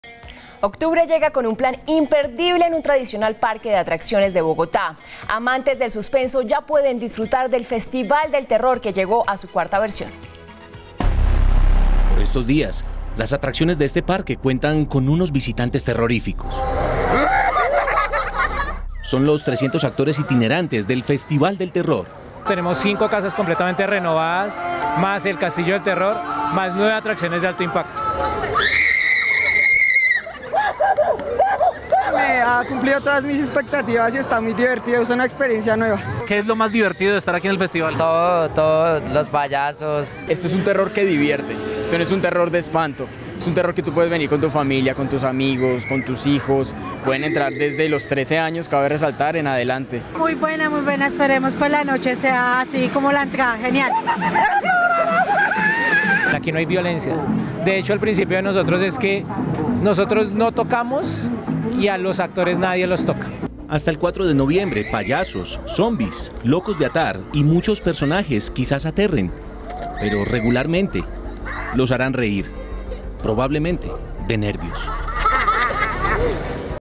RED+ | Vuelve el festival del terror a Salitre Mágico
Octubre llega con un plan imperdible en un tradicional parque de atracciones de Bogotá... amantes del suspenso ya pueden disfrutar del festival del terror que llegó a su cuarta versión...